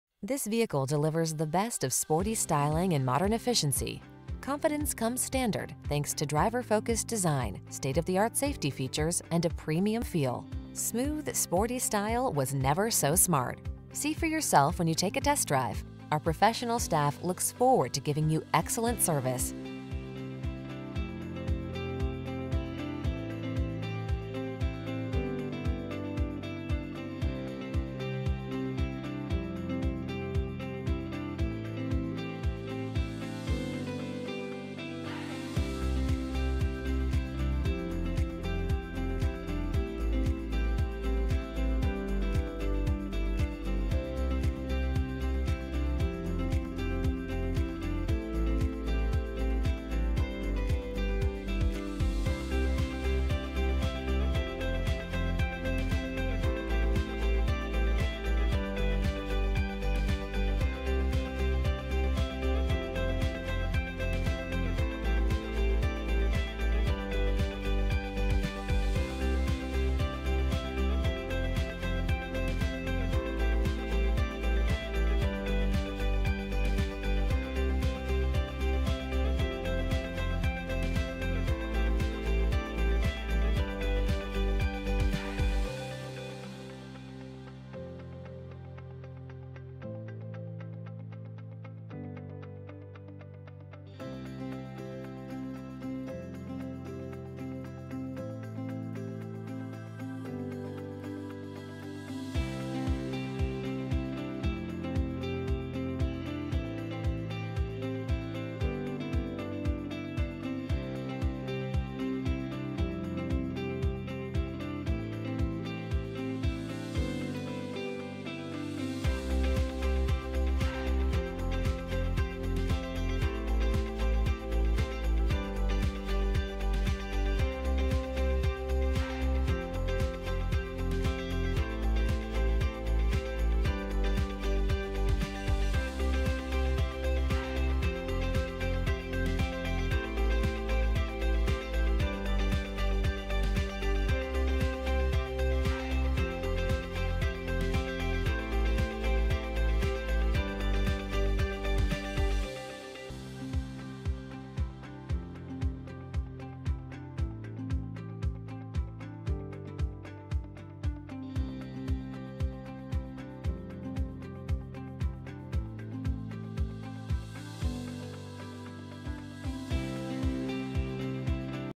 2022 Hyundai Santa Cruz Carson City, Lake Tahoe, NV HY1985
Phantom Black [LISTING TYPE] 2022 Hyundai Santa Cruz available in Carson City, Nevada at Carson Car Center. Servicing the Lake Tahoe, NV area.
2022 Hyundai Santa Cruz SEL - Stock#: HY1985 - VIN#: 5NTJCDAE0NH006017

2022 Hyundai Santa Cruz SEL SEL 8-Speed Automatic with SHIFTRONICbrbr2.5L I4 AWD 4D Crew Cab